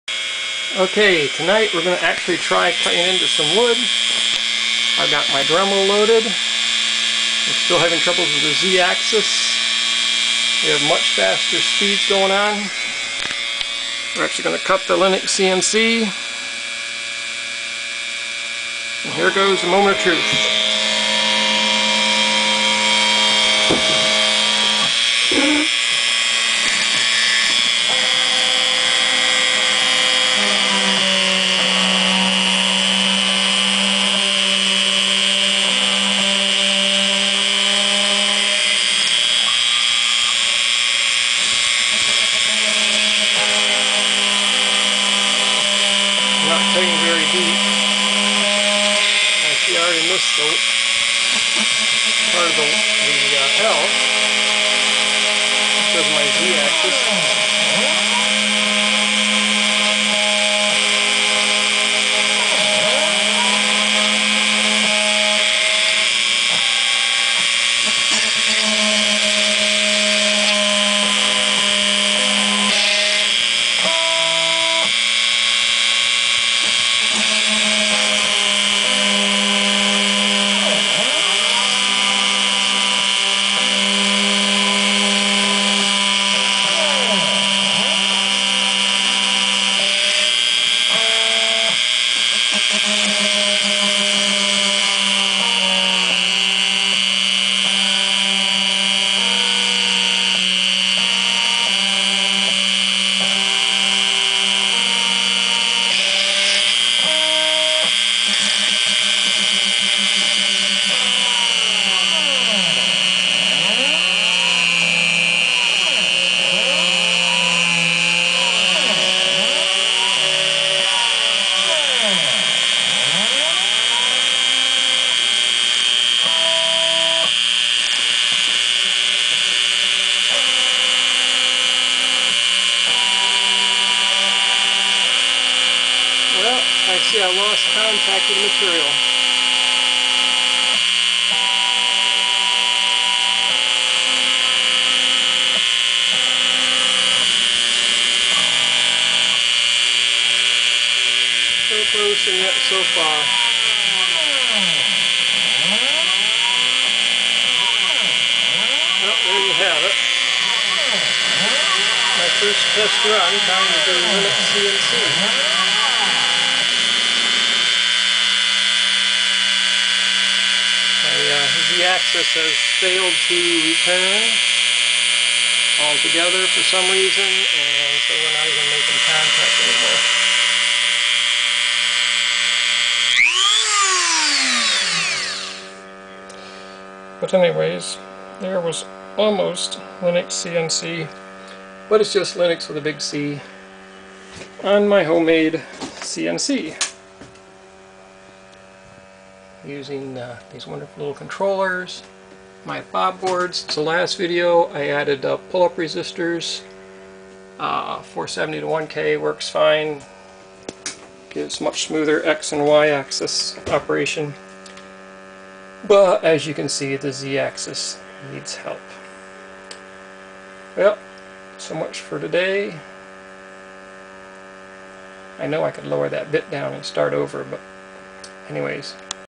DIY CNC Machine Test Number 5 LinuxCNC
This is the next test of my home made CNC as a mill.  I have improved the speed and performance and actually put a bit in the Dremel.  However the bit came loose during testing because it was too far out to begin with!